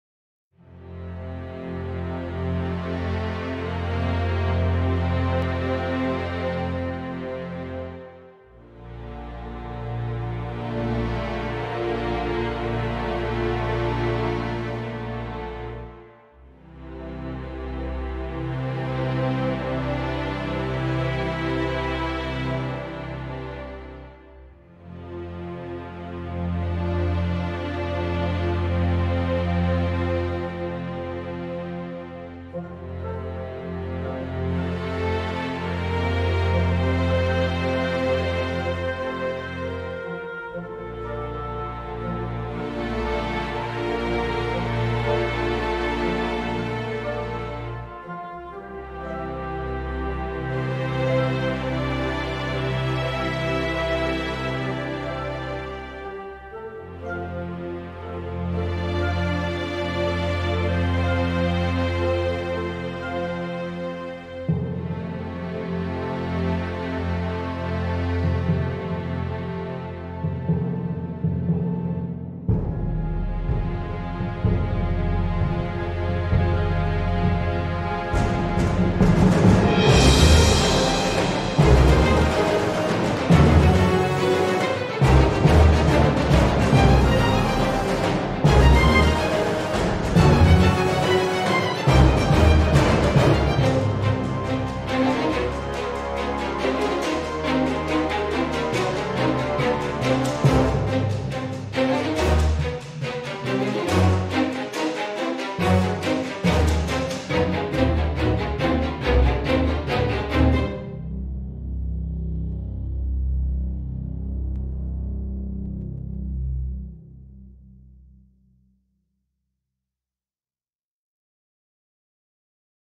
Music Weeklies Challenge - Opposing Emotions
My entry for the Music Weeklies Challenge. I tried to combine tranquillity with anxiety.